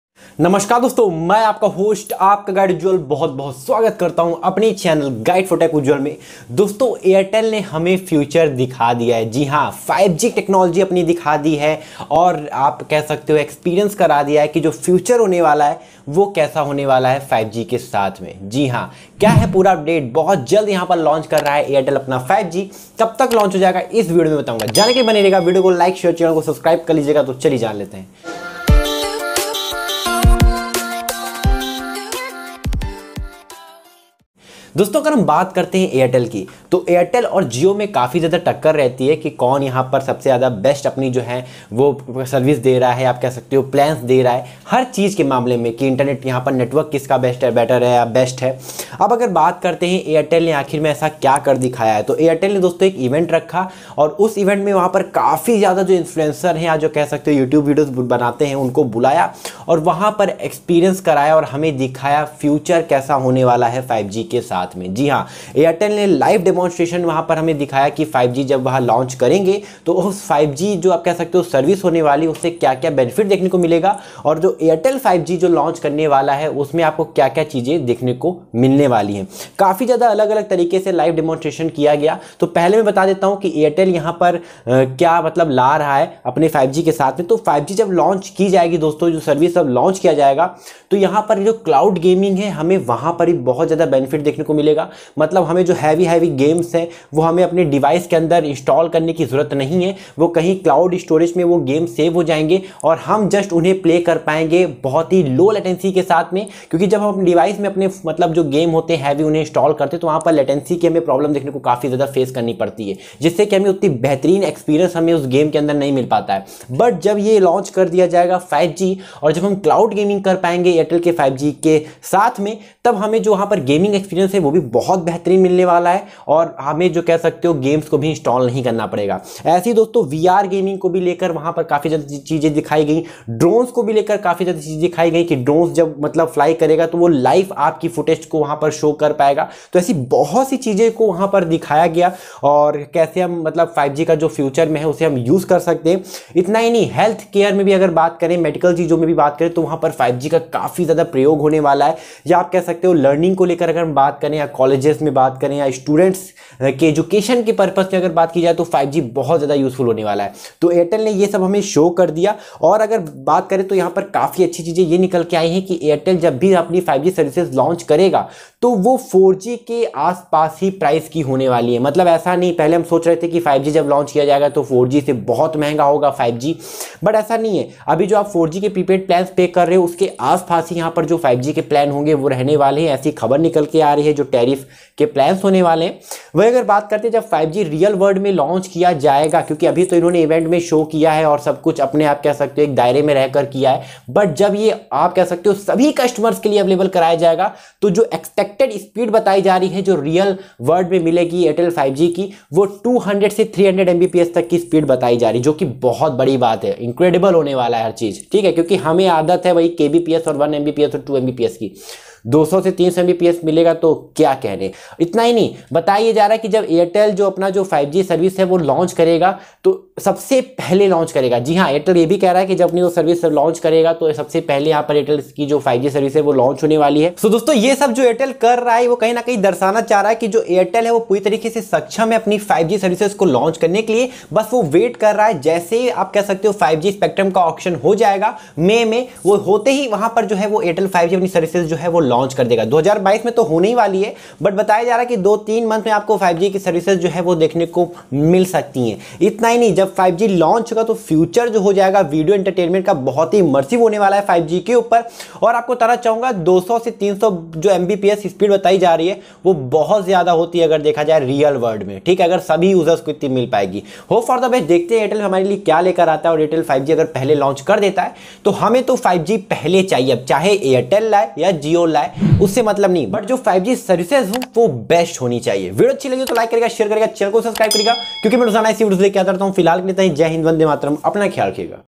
Airtel 5G की बड़ी खुशखबरी - Airtel 5G Launch Date, 5G Plan Cost, Airtel 5G Speed | 5G In 2 Months
Airtel 5G की बड़ी खुशखबरी - Airtel 5G Launch Date, 5G Plan Cost, Airtel 5G Speed For All | Airtel 5G In 2 Months

Bharti Airtel showcased 5G’s high-speed, low latency capabilities to transform the future of video entertainment and take the user experience to the next level. It is expected that an auction for 5G spectrum would be conducted within the next two months and the formal launch of the service later this year - possibly around Independence Day.

Guide4Tech Ujjwal.. :) :)